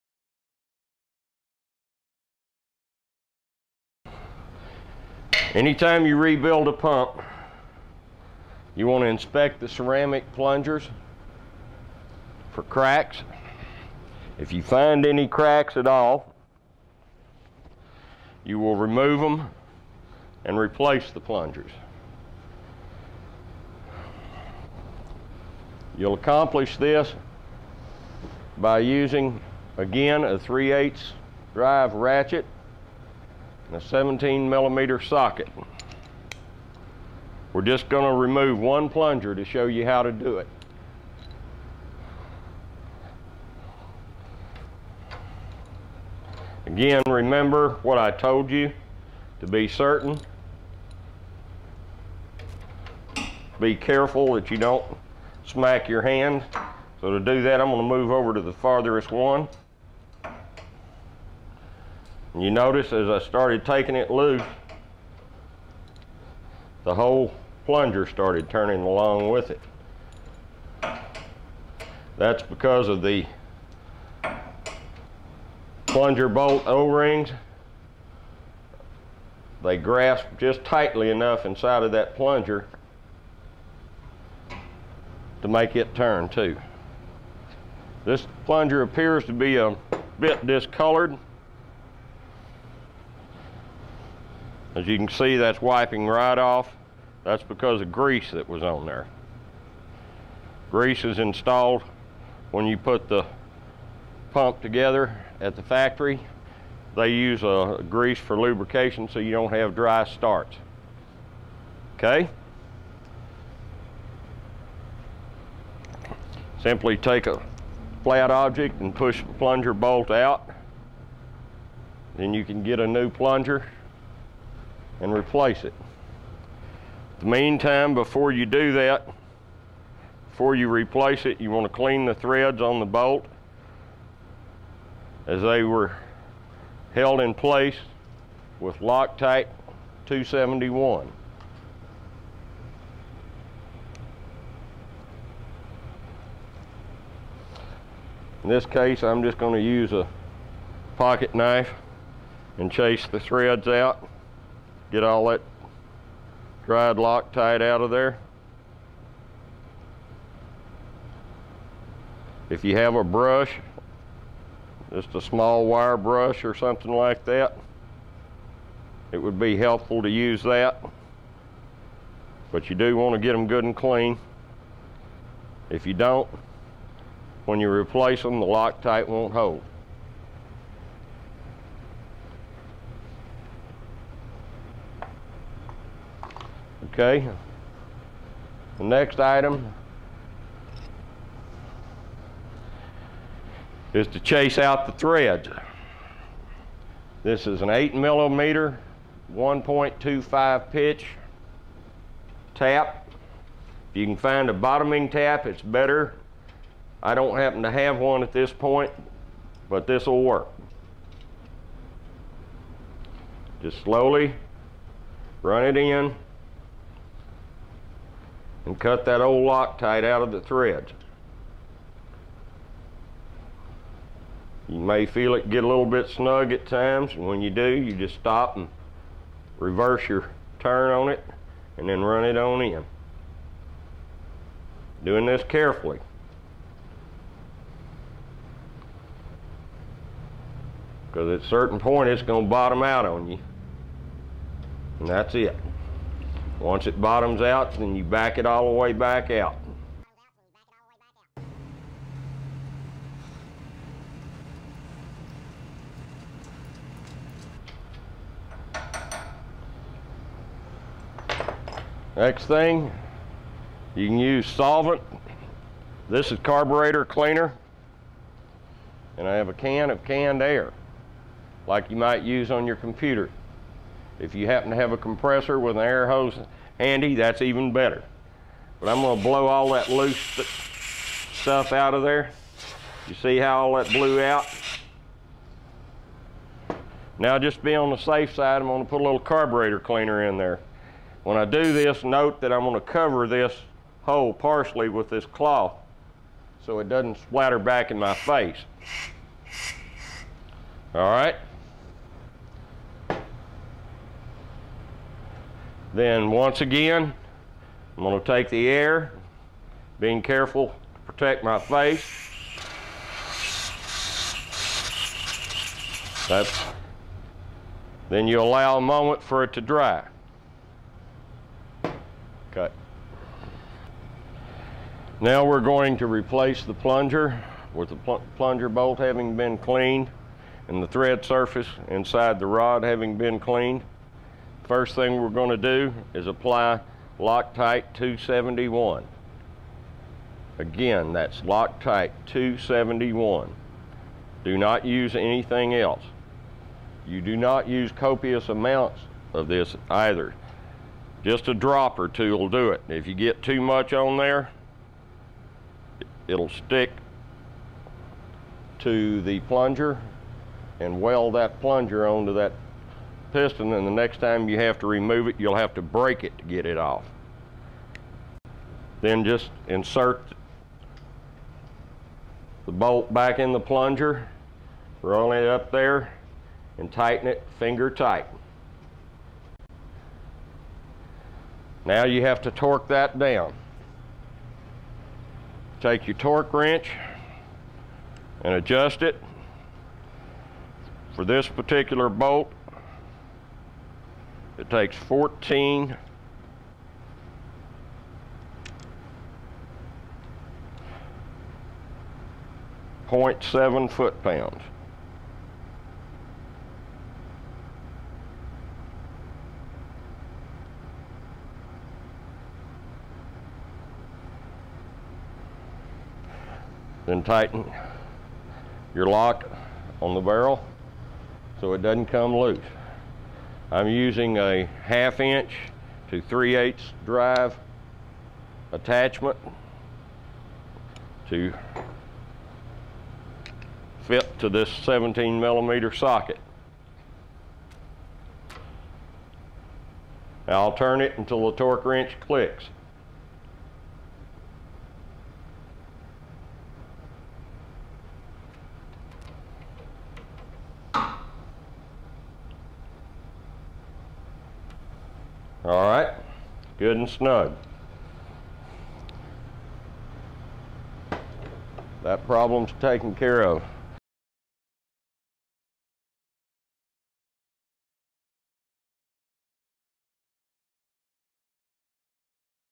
Rebuilding a General Pump P2 of 4
In this video, you'll watch how-to rebuilding a General Pump P2 of 4. Larry Hinkley with PowerWash.com describes in detail how to rebuild a General Pump

As a standard in the pressure washing industry, the general pump pressure washer pump boasts a solid shaft-forged brass manifold with an exclusive lifetime warranty. It also includes heavy-duty tapered roller bearings and oversized connecting rods for maximum life.

Anytime you rebuild a pump. You want to inspect the ceramic plungers for cracks. If you find any cracks at all. You will remove them and replace the plungers. You'll accomplish this by using again a 3/8 drive ratchet and a 17-millimeter socket.

We're just going to remove one plunger to show you how to do it. You can get a new plunger and replace it meantime before you do that before you replace it.

Before putting the plunger on the pump machine then you need to brush plunger and make clean. When you replace them the Loctite won't hold.

I hope you enjoy and learn something new from this rebuilding a general pump video.